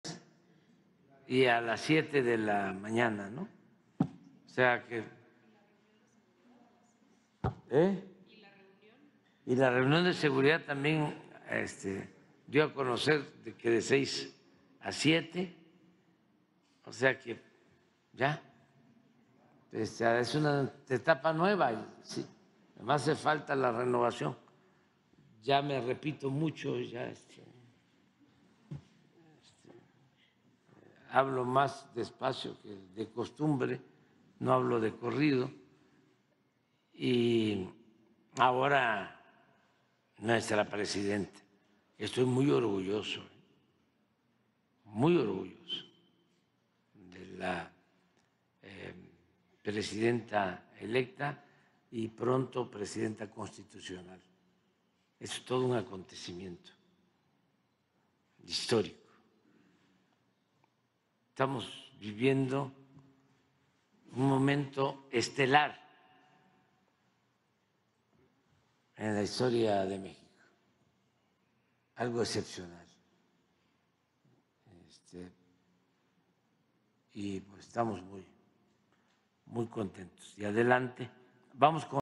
AMLO, muy contento porque SHEINBAUM continuarÁ con las mañaneras y reunión de SEGURIDAD
El presidente lópezobrador dijo que ayer se enteró que la Dra. claudiasheinbaum continuará con las conferencias mañaneras, así como con la reunión de seguridad. El mandatario dijo que es una etapa nueva, que hace falta la renovación y reiteró que está muy orgulloso de la presidenta electa y próxima presidenta constitucional.